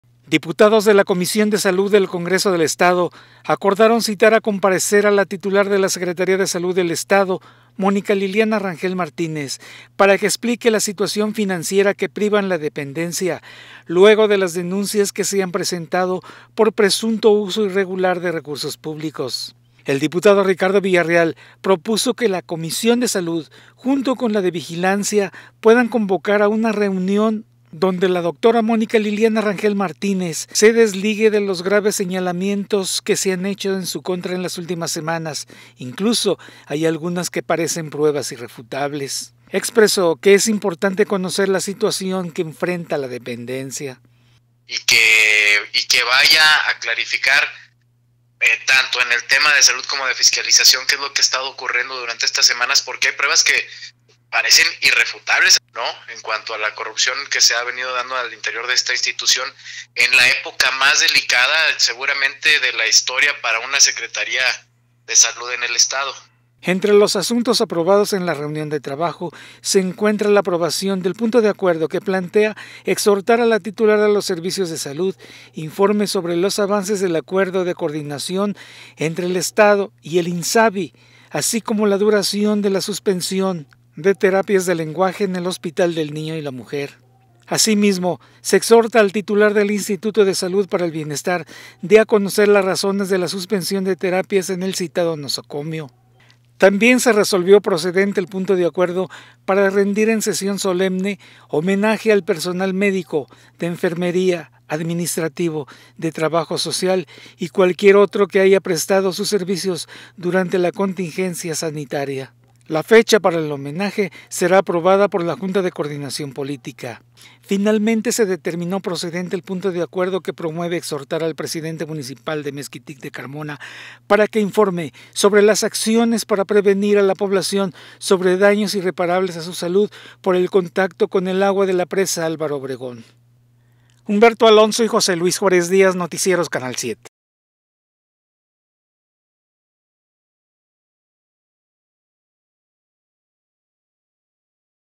Rangel Martínez deberá explicar situación financiera de la Secretaría de Salud ante diputados.
SLP.- Los diputados de la Comisión de Salud del Congreso del Estado acordaron citar a comparecer a la titular de la Secretaría de Salud del Estado, Mónica Rangel Martínez, para que explique la situación financiera que priva en la dependencia, luego de las denuncias que se han presentado por presunto uso irregular de recursos públicos.

El diputado Ricardo Villarreal Loo, propuso que junto con la comisión de vigilancia se puedan convoca a una reunión con la titular de la dependencia para se desligue de los graves los señalamientos que se han hecho en las últimas semanas, incluso hay algunas que parecen pruebas irrefutables.

Expresó que es importante conocer la situación que enfrenta esta dependencia.

Entre los asuntos aprobados en la reunión de trabajo, se encuentra la aprobación del Punto de Acuerdo que plantea exhortar a la titular de los Servicios de Salud, informe los avances del acuerdo de coordinación este el Estado e INSABI; así como la duración de la suspensión de terapias de lenguaje en el Hospital del Niño y la Mujer.

Asimismo, exhortar al Titular del Instituto de Salud para el Bienestar, de a conocer las razones de la suspensión de terapias en el citado nosocomio.

También se resolvió procedente el Punto de Acuerdo, para rendir en Sesión Solemne homenaje al personal médico, de enfermería, administrativo, de trabajo social, y cualquier otro que haya prestado sus servicios durante la contingencia sanitaria del año 2020; la fecha en específico será aprobada por la Junta de Coordinación Política.

Se determinó procedente el Punto de Acuerdo que promueve exhortar al Presidente Municipal de Mexquitic de Carmona, informar acciones para prevenir a la población sobre daños irreparables a su salud, por contacto con agua de la presa Álvaro Obregón y cómo atiende la contaminación de dicho embalse.